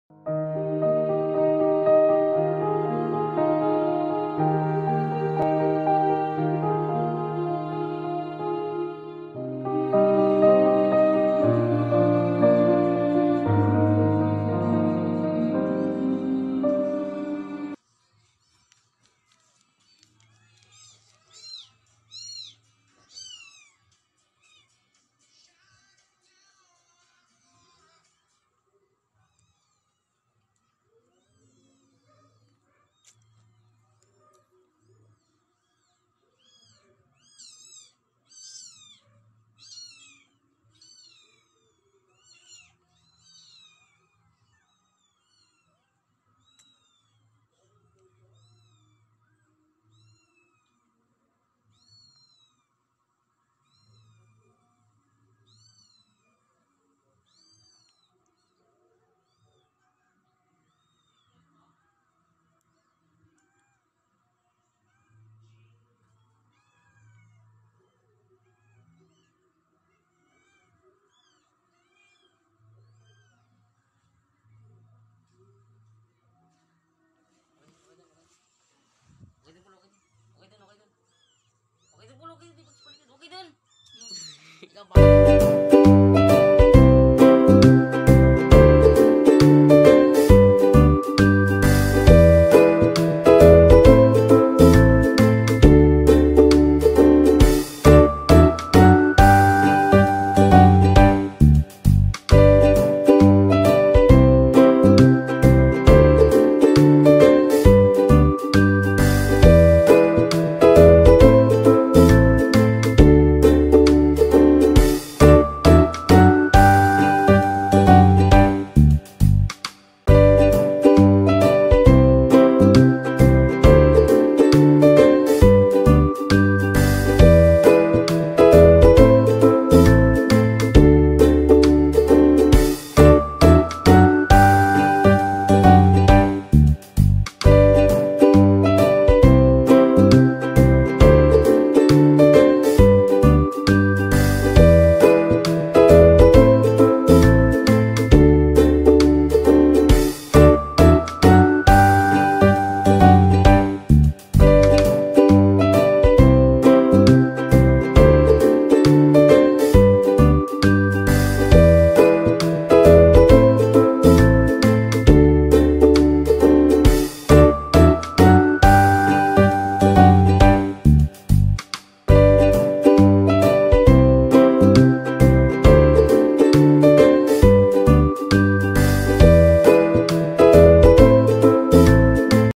nakakagigil kasi 😺😹
ilovecat iloveanimals just miss this moment taking care cats and kittens..